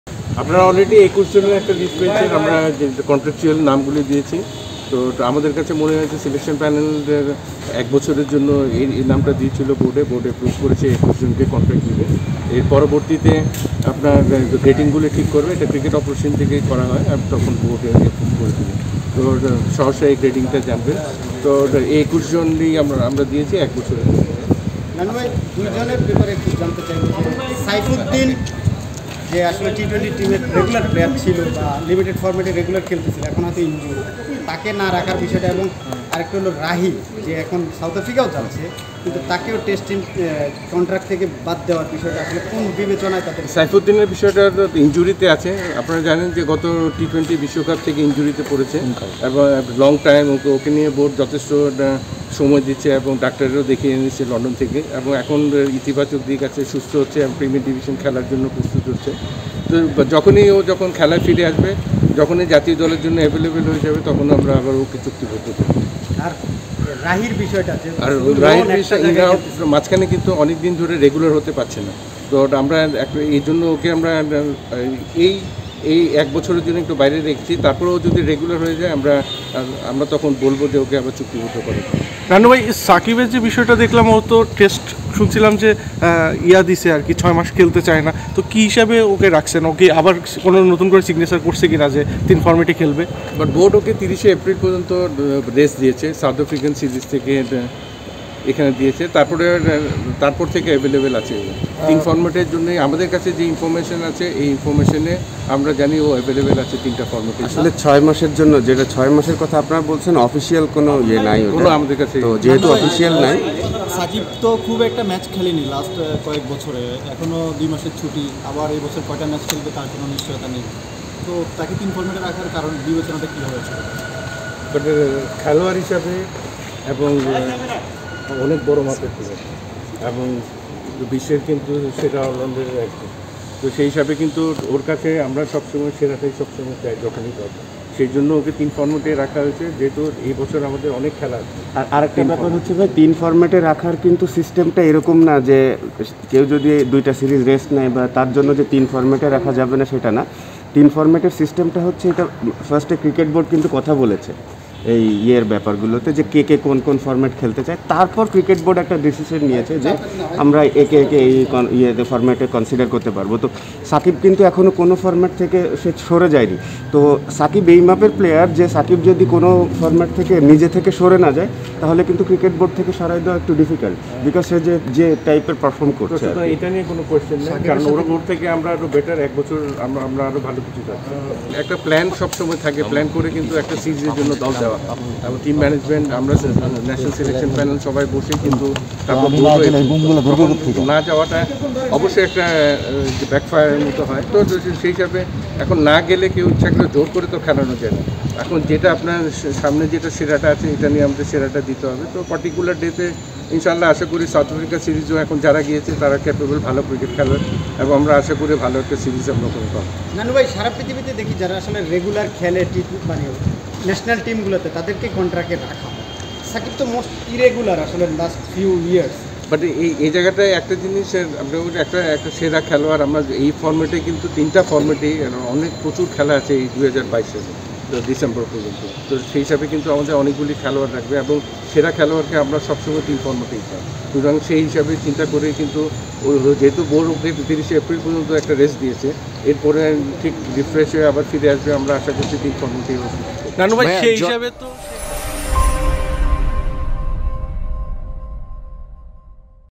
সাকিবকে তিন ফরম্যাটের চুক্তিতে রাখার কারণ কী | shakib al hasan | BCB | Dhaka Post
জাতীয় দলের কেন্দ্রীয় চুক্তির তালিকা প্রকাশ করেছে বাংলাদেশ ক্রিকেট বোর্ড। যেখানে তিন ফরম্যাটেই জায়গা পেয়েছেন সাকিব আল হাসান।